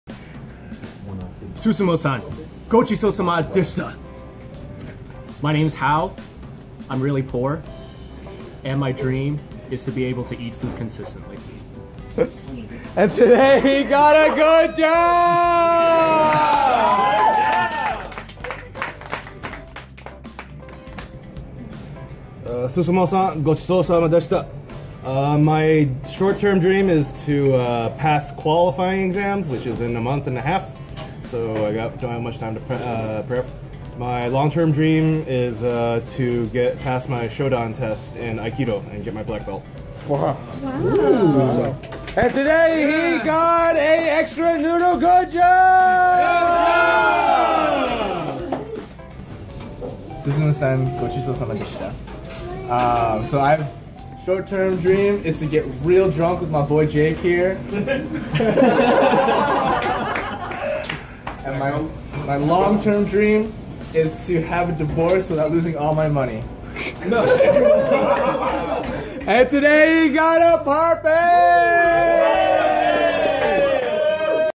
Yume Wo Katare: Susumu Ohata
Customers thanking Susumu Ohata for free ramen!

Yume Wo Katare held a crowdfunding campaign to support Boston students in achieving their dreams!